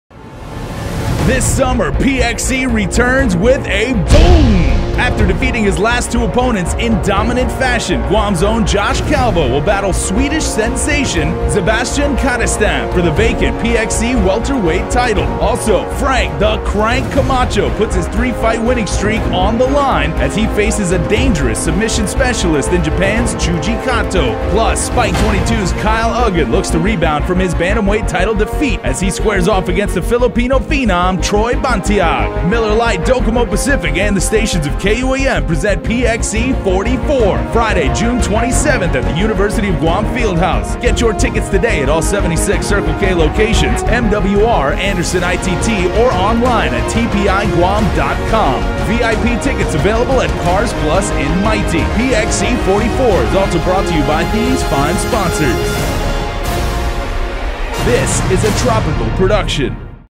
PXC 44 | Official Trailer #1
COME JOIN THE ACTION!!!

Pacific Xtreme Combat 44
June 27th, 2014
Main Event: Kadestam vs Calvo
at the UOG Calvo Field House, Mangilao Guam

Ticket Outlets:
76 Circle K Locations
Andersen ITT
MWR
Crowns Guam
Cars Plus (VIP)